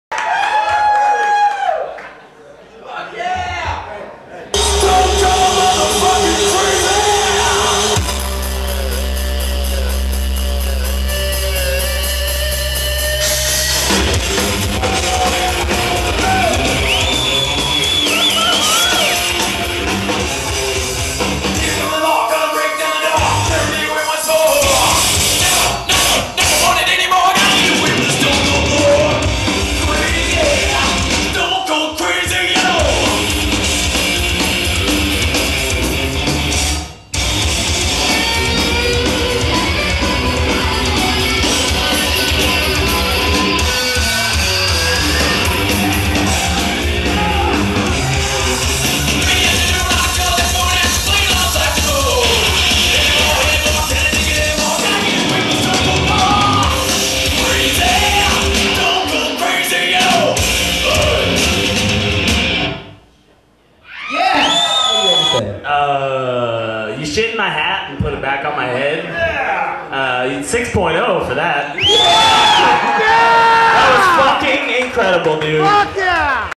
US Air Guitar Regional - Des Moines - Rocktopuss Round2
The Rocktopuss makes a mess of judge Dingo Watson's hat in Round 2.